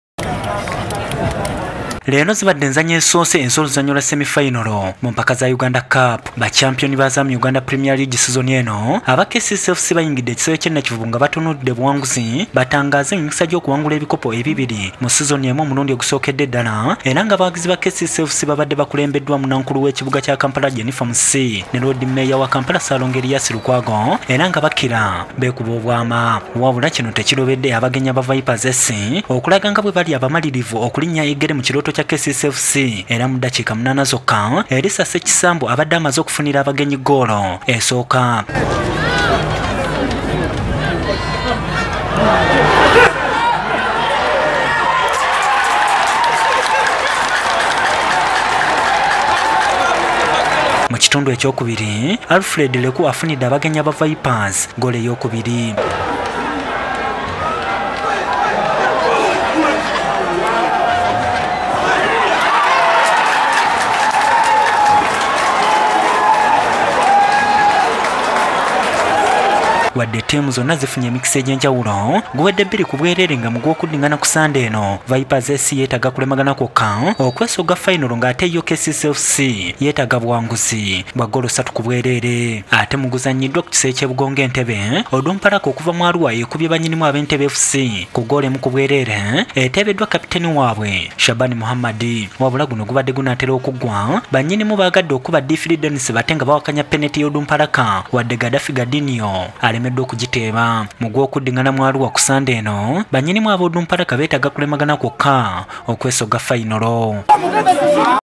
Semi fayinolo: KCCA FC 0-2 Vipers SC
Mu gw'okudding'ana Vipers yeetaaga maliri.